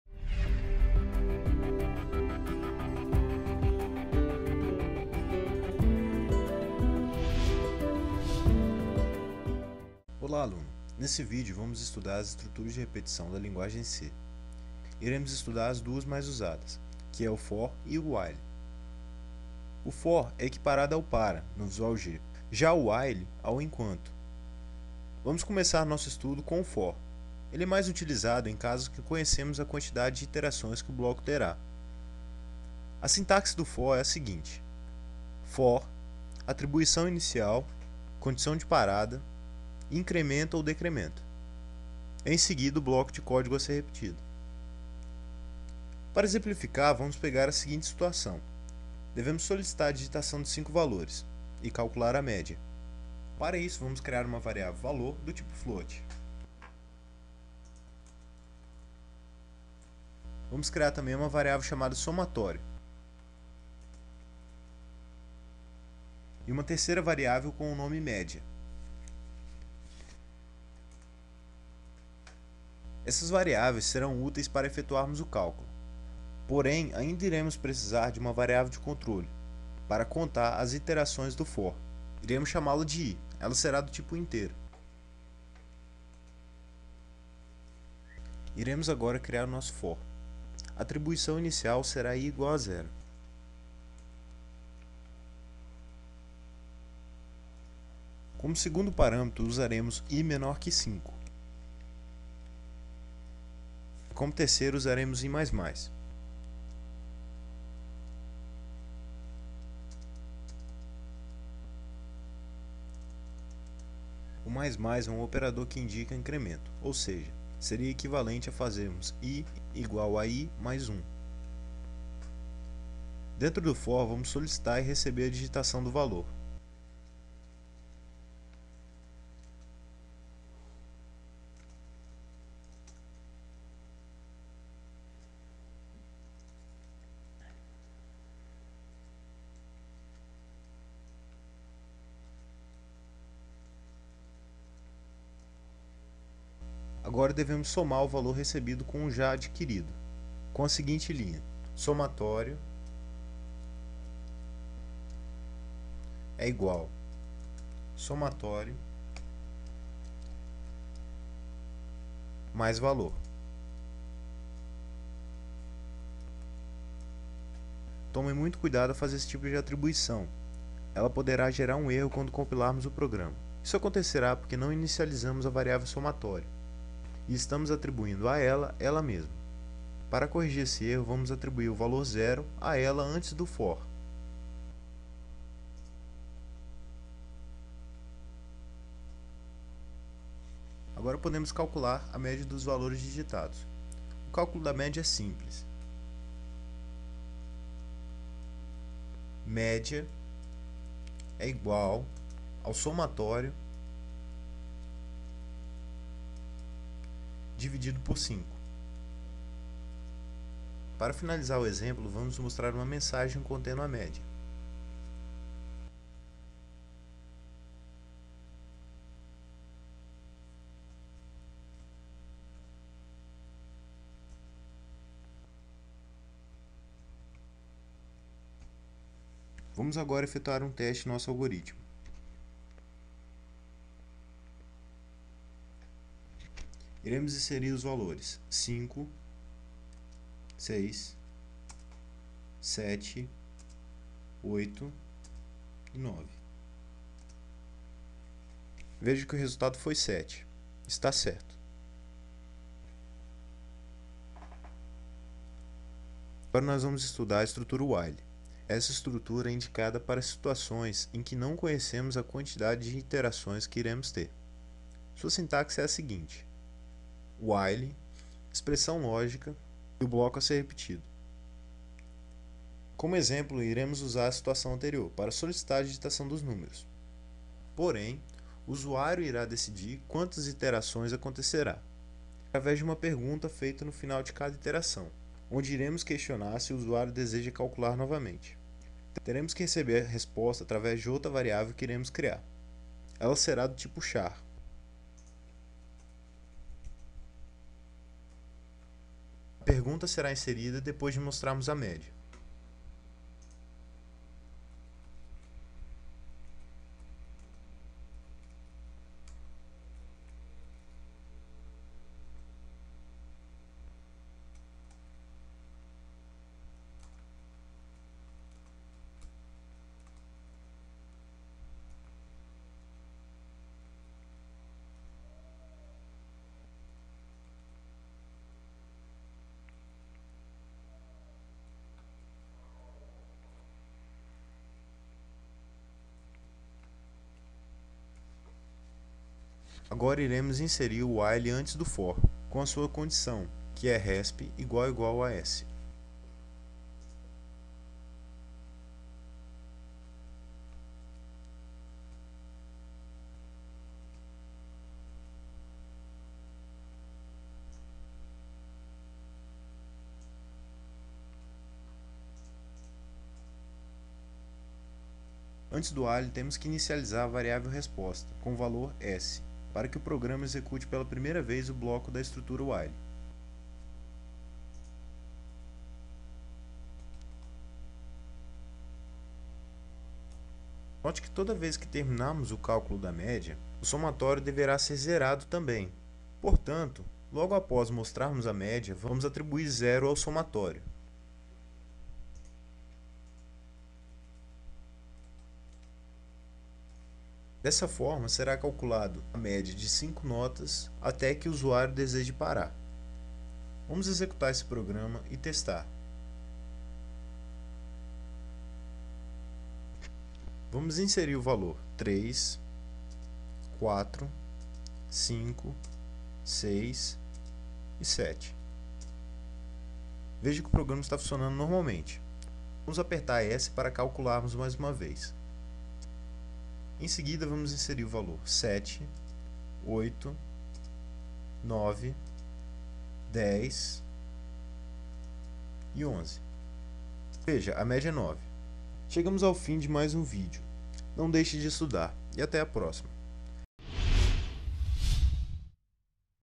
Tutorial - Estruturas de Repetição As duas mais usadas - Lógica de Programação
Curso: Técnico em Informática
Disciplina: Lógica de Programação
Professor: Aílton Duarte